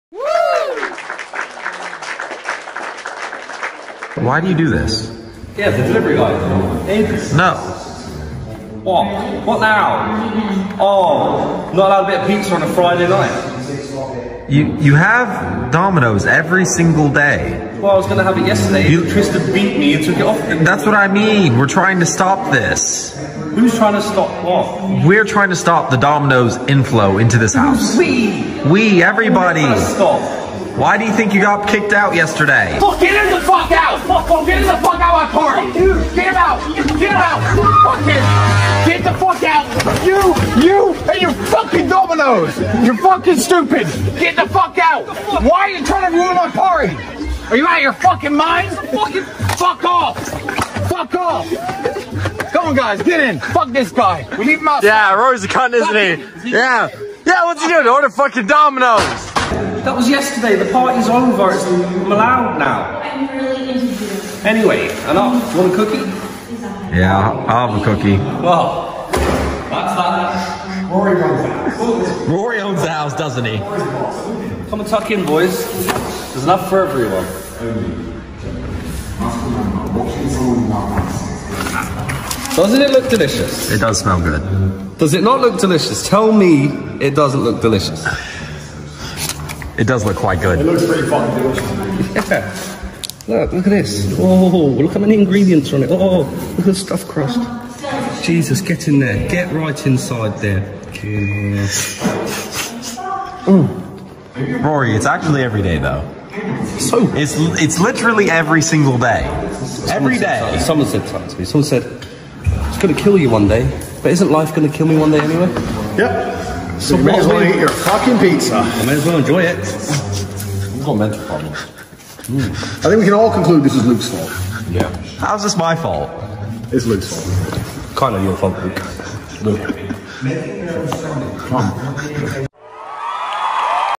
Why you should never ruin Tristan Tate’s party by ordering Dominoes 🤣🤣🤣
Credit to Andrew Tate and Tristan Tate

The original video is on Tate speech YouTube channel

This is why you should never order domino’s at Tristan Tate’s party